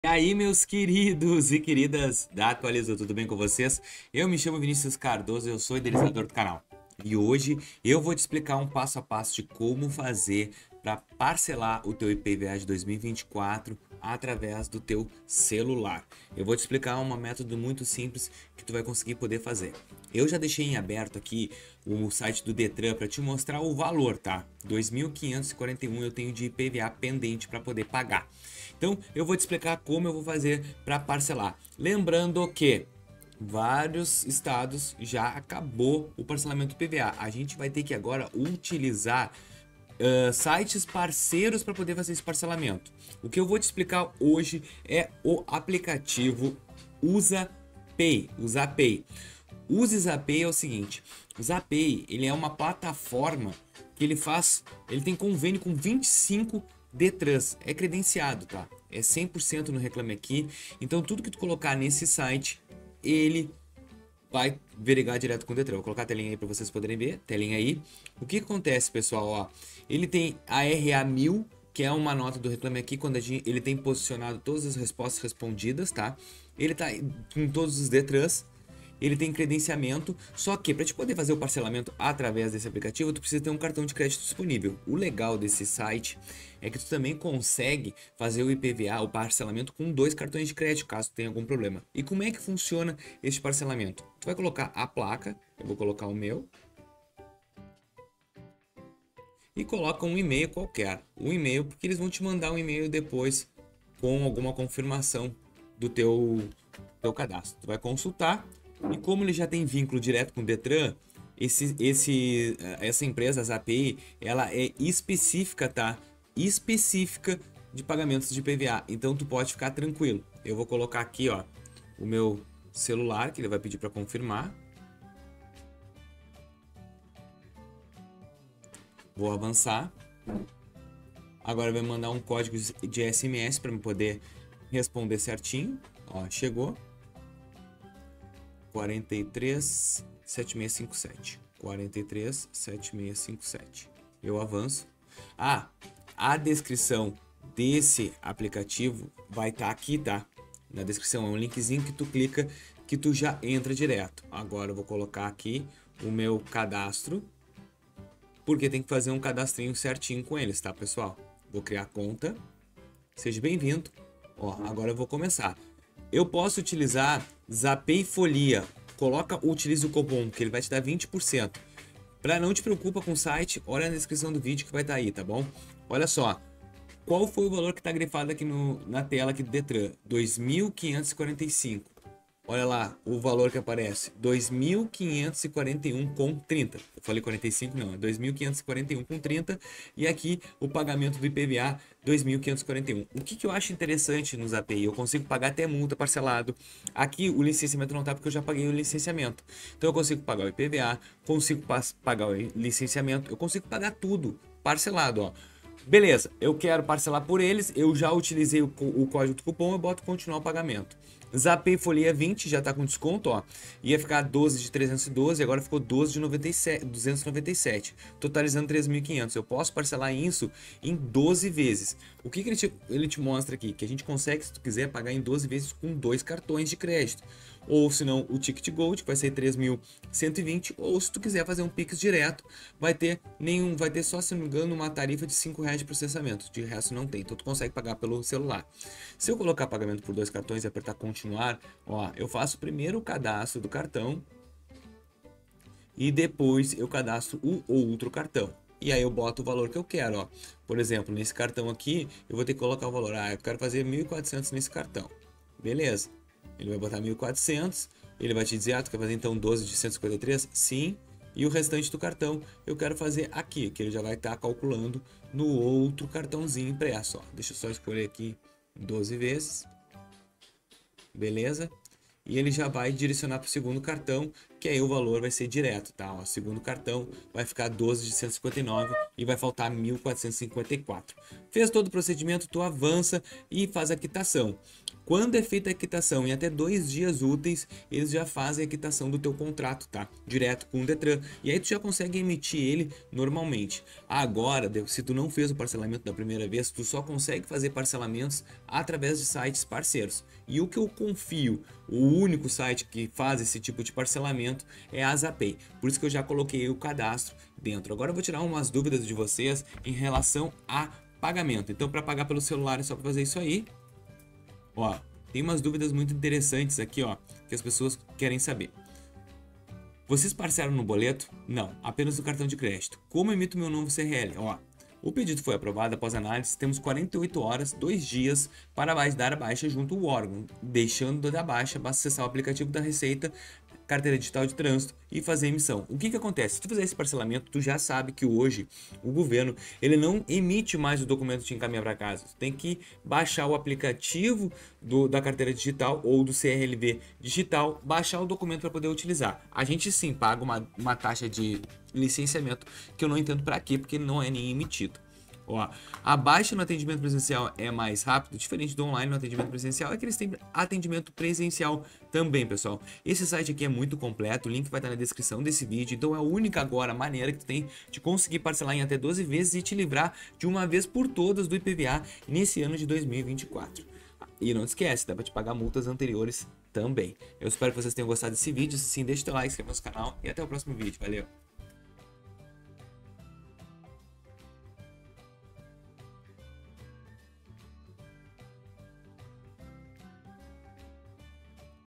Como PARCELAR o IPVA 2024 pelo CELULAR
Agora ficou mais fácil parcelar o IPVA 2024 diretamente pelo celular com o Zapay! Neste vídeo, vou te guiar por todo o processo, passo a passo, para parcelar o seu IPVA de forma rápida, segura e conveniente.

Direto ao Ponto:
00:00 - Início
01:20 - Como PARCELAR o IPVA 2024 pelo CELULAR
06:11 - Encerramento